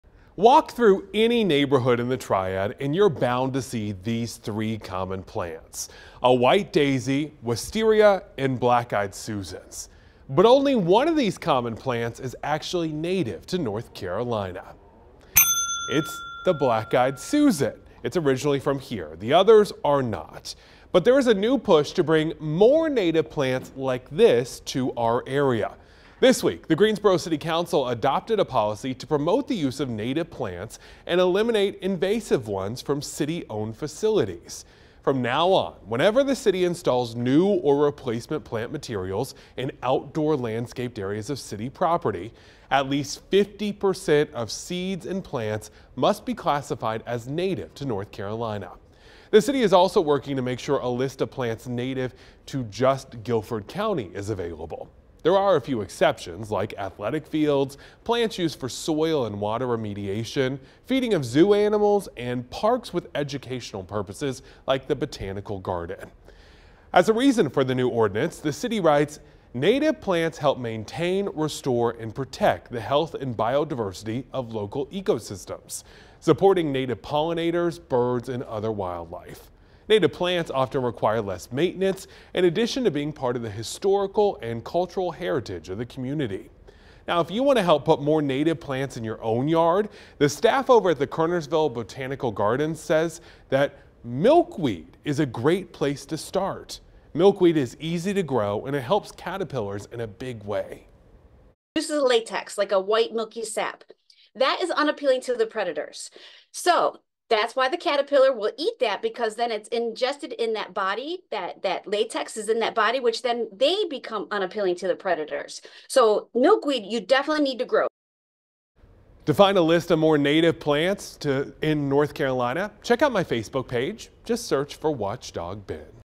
Greensboro wants more native plants to protect the environment. Here’s how you can help…
The City just passed new rules for planting on city property. There’s one flower you can add to your garden to make a big impact!